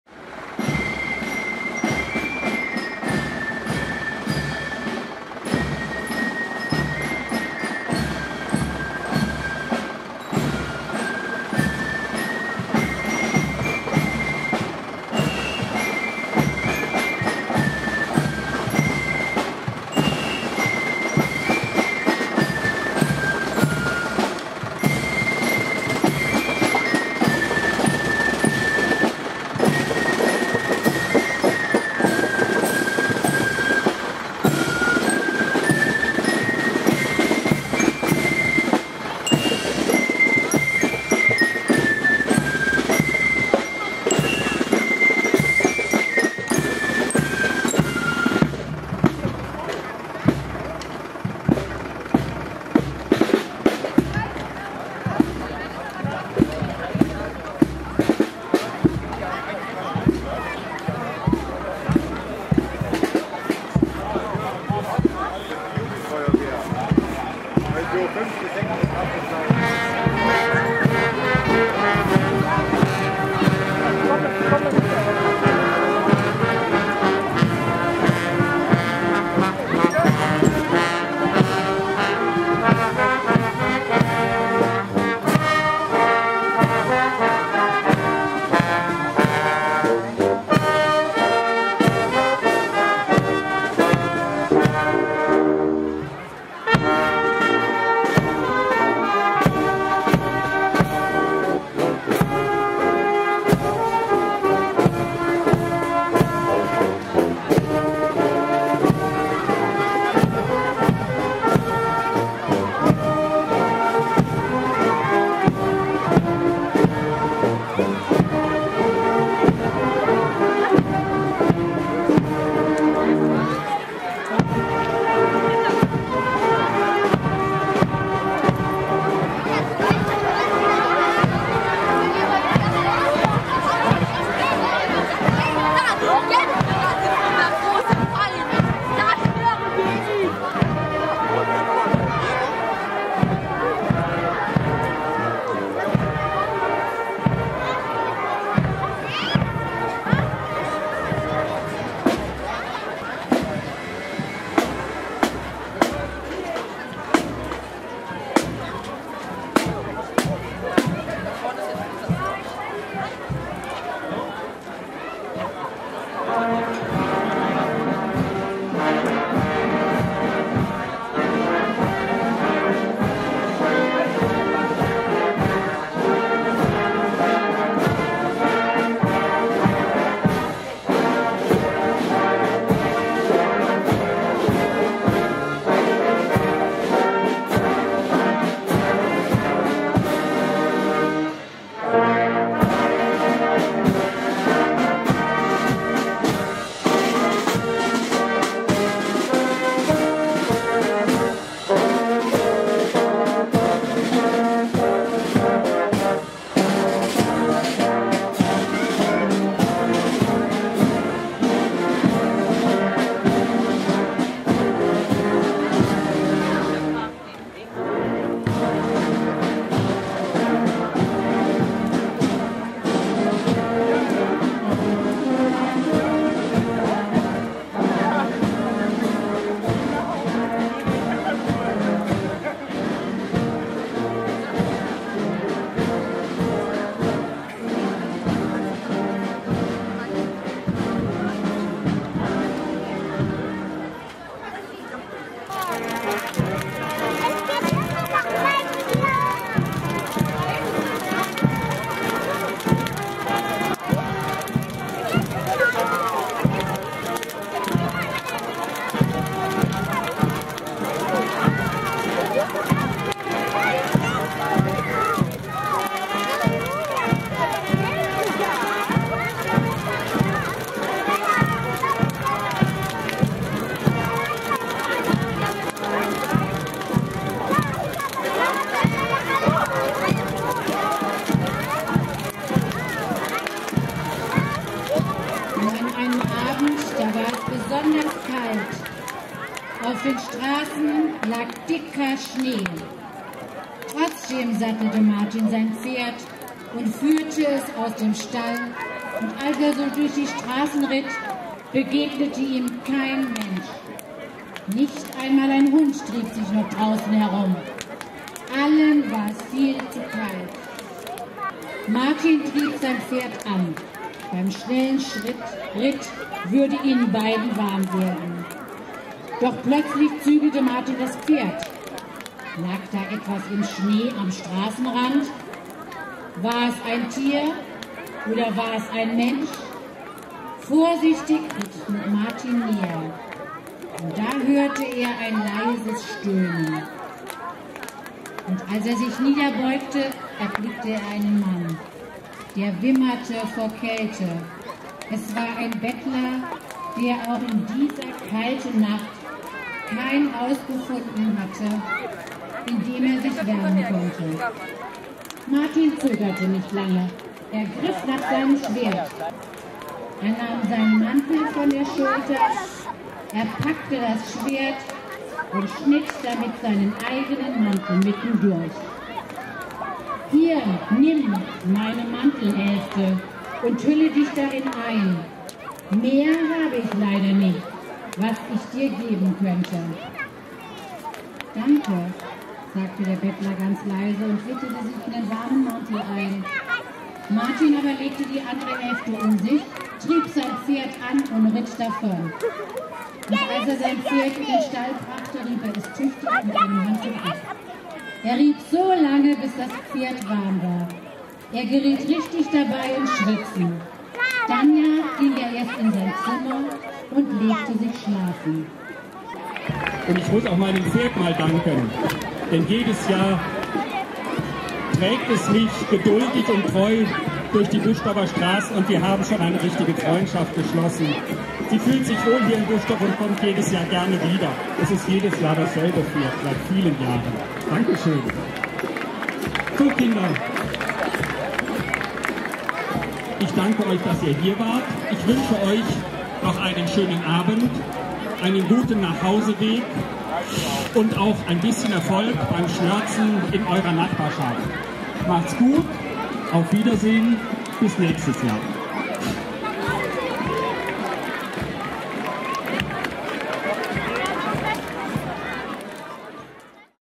Sankt Martin in Buschdorf 2012
Veranstaltet vom Ortsfestausschuss Buschdorf e.V. in Zusammenarbeit mit der Freiwilligen Feuerwehr Buschdorf